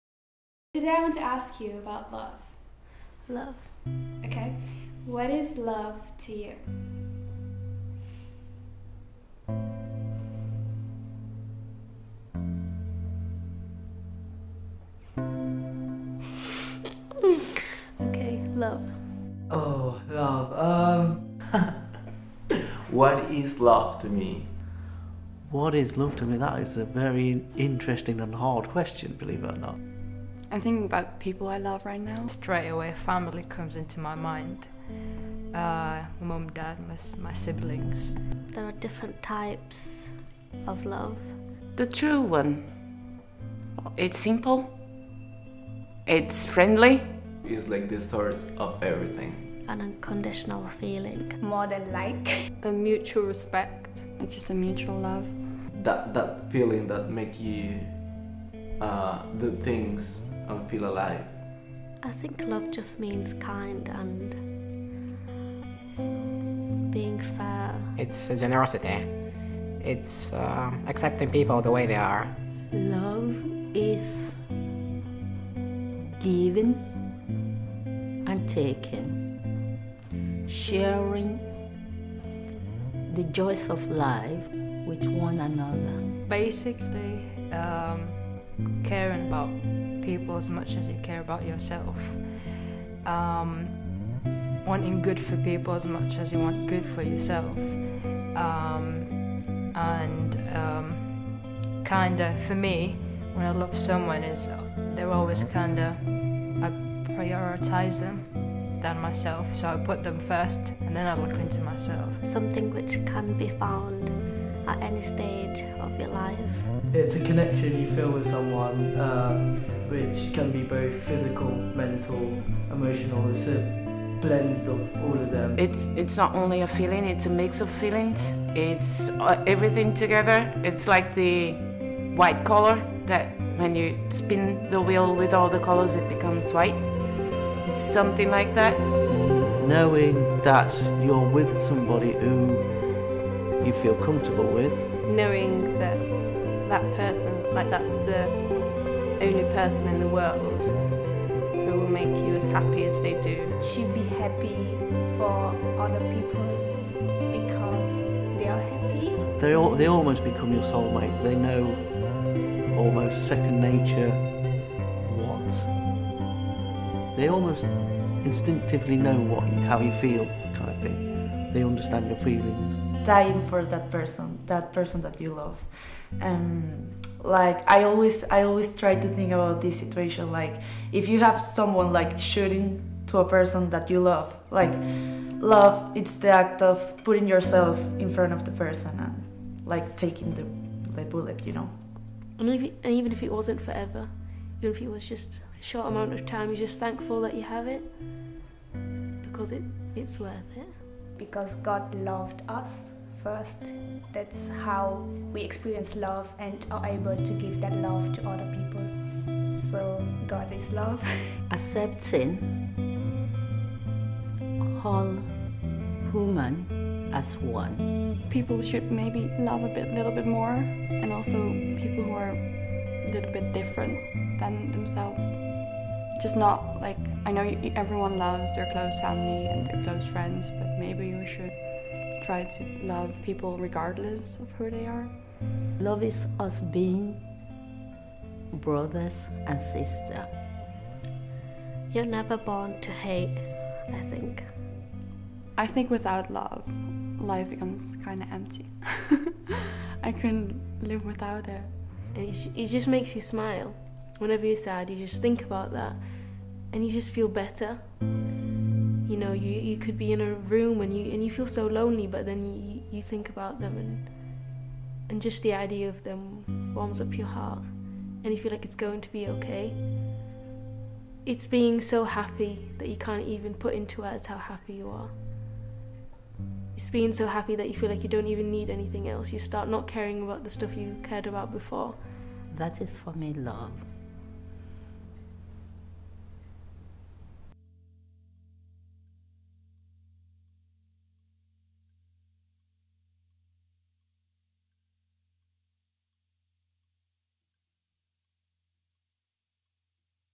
What is LOVE to you?
An independent project where I try to explore the meaning of love.

Directed and produced by: Mónica Costa